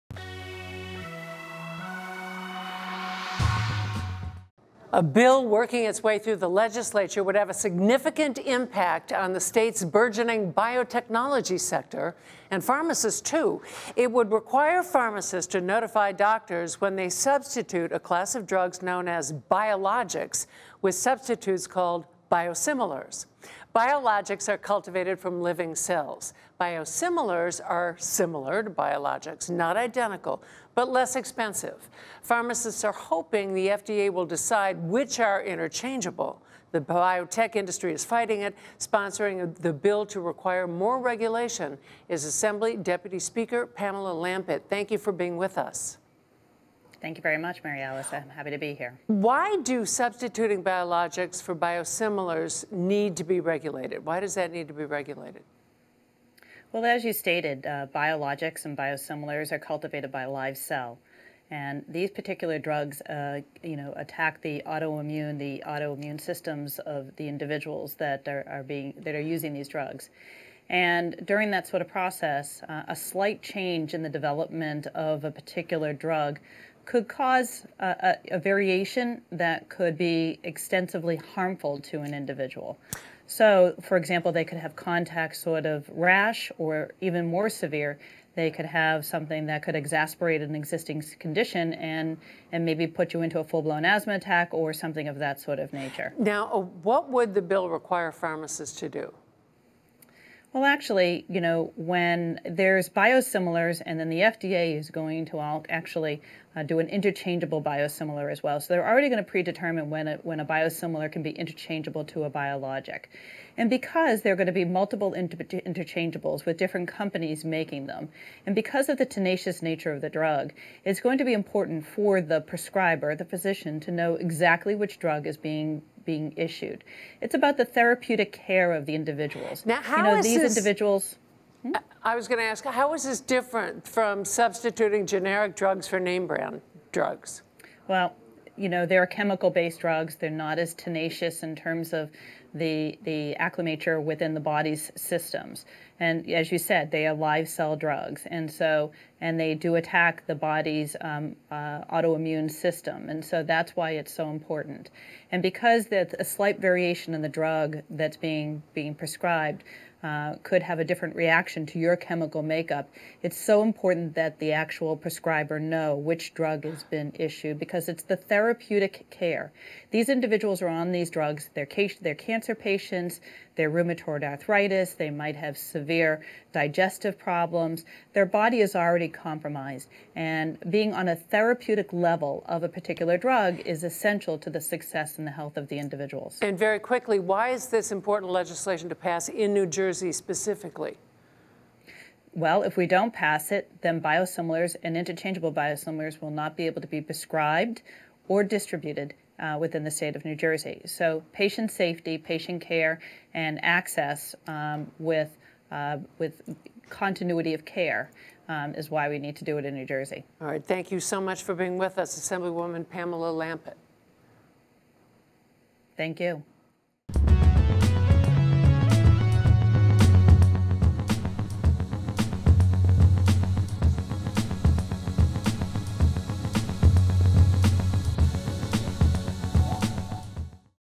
Asw. Lampitt: NJ Should Regulate Biosimilar Drugs
Assemblywoman Pamela Lampitt says that a bill requiring pharmacists to notify doctors when they substitute biologics with biosimilars should pass to increase patient safety.